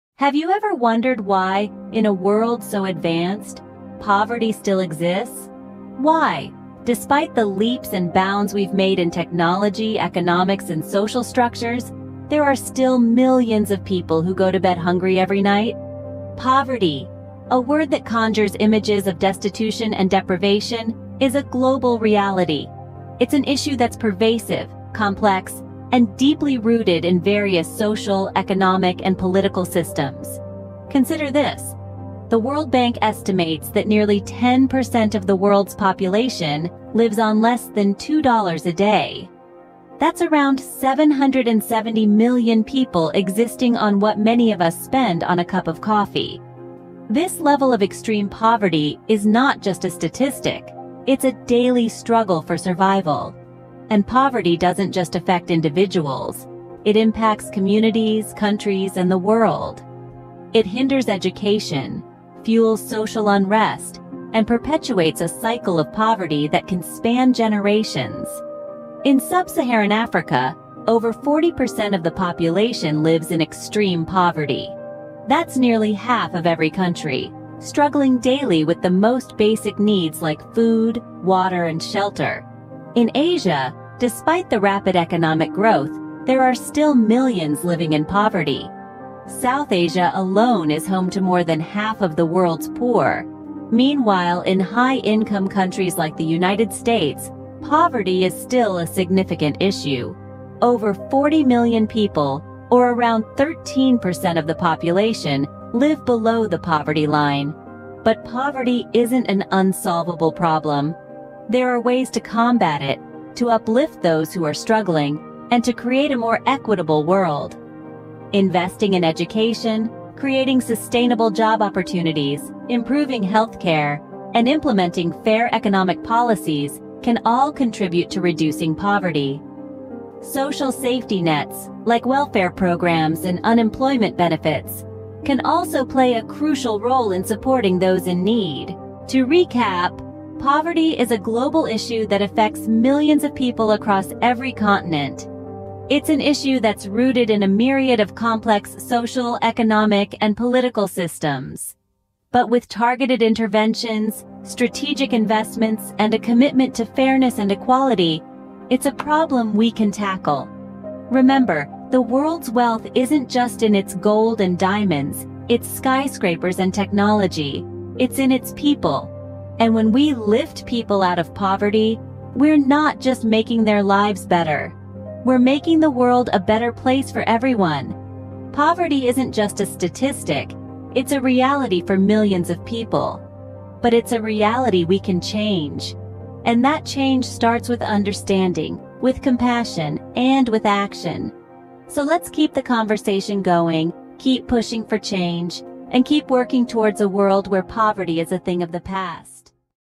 Unveiling Poverty: The World's Biggest Issue
In this thought-provoking video, we tackle the pressing issue of poverty - one of the world's biggest challenges. Join us as we deep dive into the complexities surrounding this global issue and explore potential solutions.

Poverty affects millions of people worldwide, and its ramifications are vast. By understanding its multifaceted nature, we can work towards finding sustainable solutions. Throughout the video, we'll shed light on the root causes, share eye-opening statistics, and highlight the experiences of those living in poverty.

Moreover, we'll examine innovative strategies and initiatives that are making a positive impact on poverty reduction. From microfinance to education and healthcare initiatives, we'll discuss how these interventions can transform lives and create a more equitable future.

By raising awareness and fostering empathy, we believe that together, we can address this urgent problem. Let's inspire change and make a difference!

OUTLINE:

00:00:00 The Global Reality of Poverty
00:02:04 The Power of Change